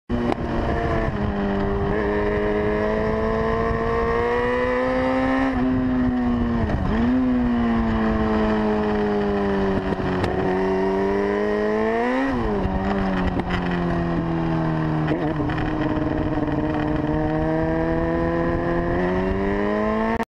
Nearly highside R6
Well maybe a little too much gas in the turn (or too early), the motorcycle loses rear...
Bike was a stock Yamaha R6 2004.